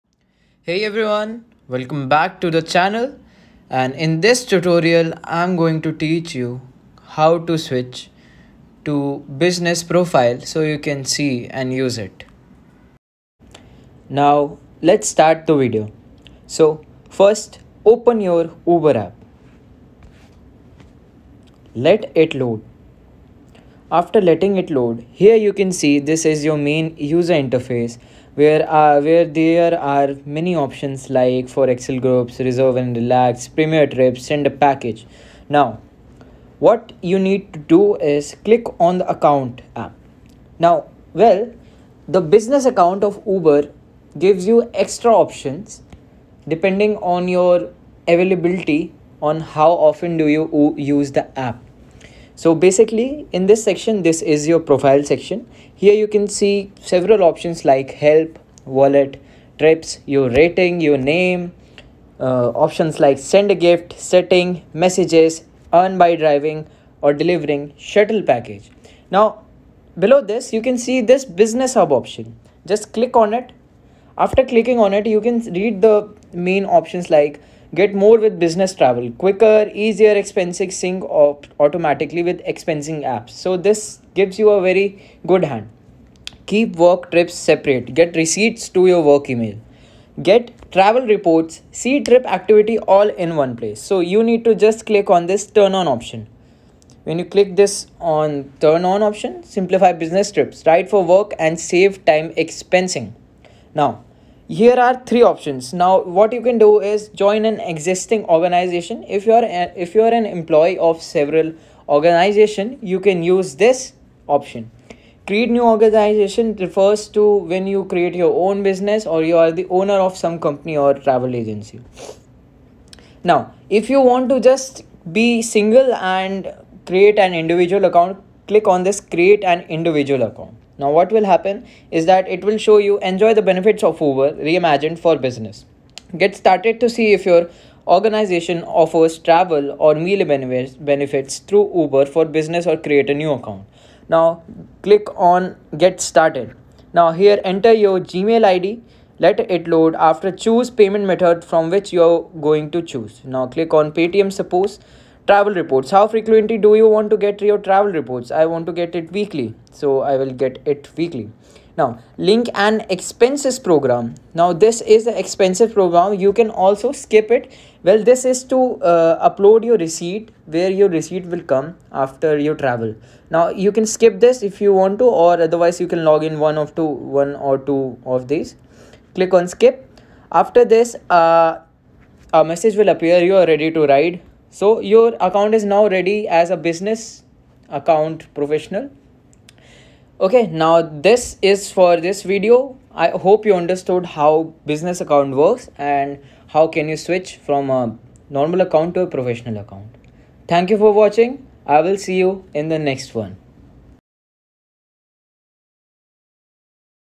How To Use Business Profile Uber Tutorial
In this video I show you How To Use Business Profile Uber Tutorial
and learn to do it in just a few minutes by following this helpful tutorial.